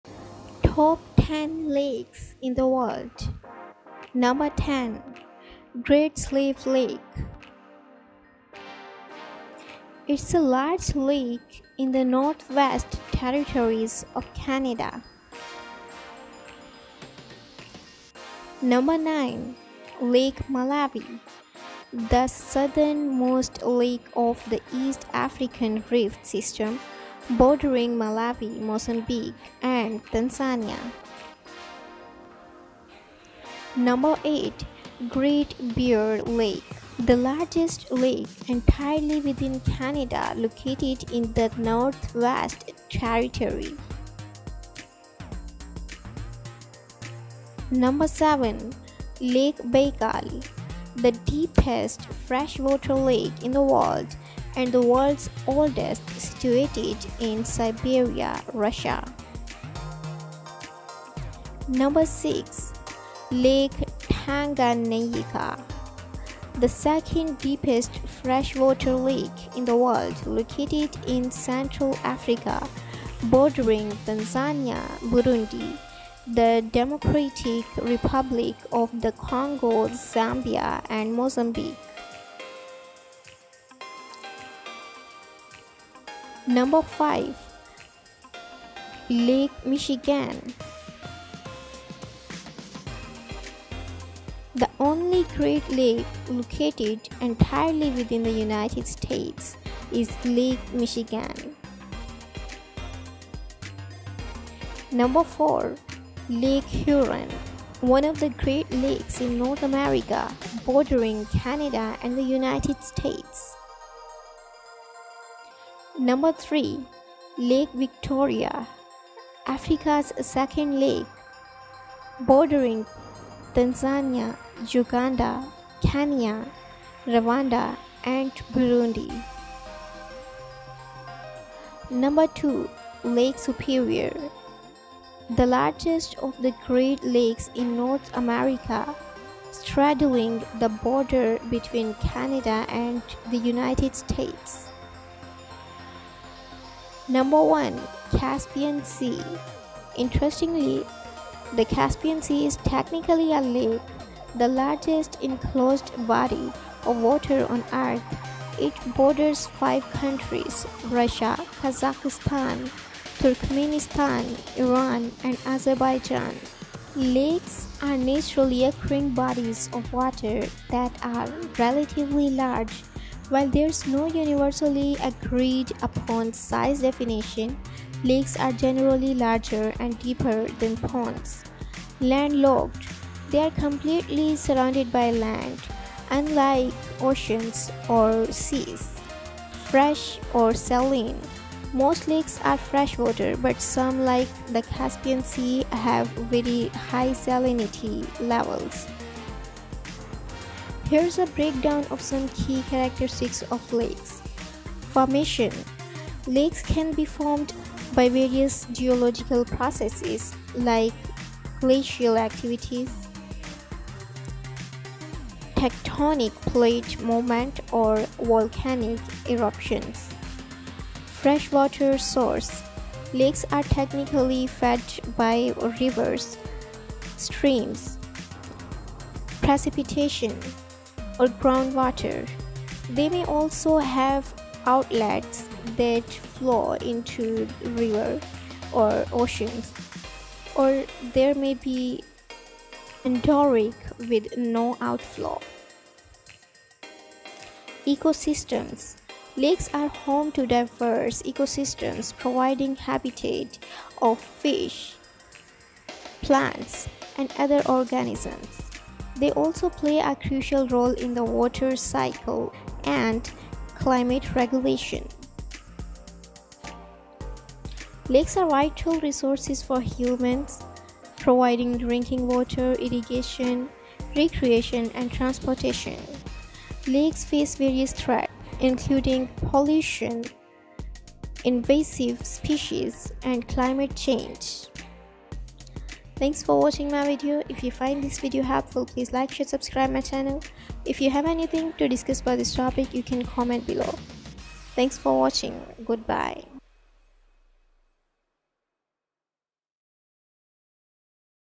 Top 10 Biggest Lakes in the world
Which great lake is the most popular? What are the top 10 biggest lakes in the world?

Caspian Sea (371,000 sq km) - Interestingly, the Caspian Sea is technically a lake, the largest enclosed body of water on Earth. It borders five countries: Russia, Kazakhstan, Turkmenistan, Iran, and Azerbaijan.
Lake Superior (82,100 sq km) - The largest of the Great Lakes in North America, straddling the border between Canada and the United States.
Lake Victoria (69,484 sq km) - Africa's largest lake, bordering Tanzania, Uganda, Kenya, Rwanda, and Burundi.
Lake Huron (59,590 sq km) - Another of the Great Lakes in North America, bordering Canada and the United States.
Lake Michigan (57,753 sq km) - The only Great Lake located entirely within the United States.
Lake Tanganyika (32,900 sq km) - The second-deepest freshwater lake in the world, located in Central Africa, bordering Tanzania, Burundi, the Democratic Republic of the Congo, Zambia, and Mozambique.
Lake Baikal (31,722 sq km) - The deepest freshwater lake in the world and the world's oldest, situated in Siberia, Russia.
Great Bear Lake (31,328 sq km) - The largest lake entirely within Canada, located in the Northwest Territories.
Lake Malawi (29,600 sq km) - The southernmost lake of the East African Rift System, bordering Malawi, Mozambique, and Tanzania.
Great Slave Lake (28,568 sq km) - Another large lake in the Northwest Territories of Canada.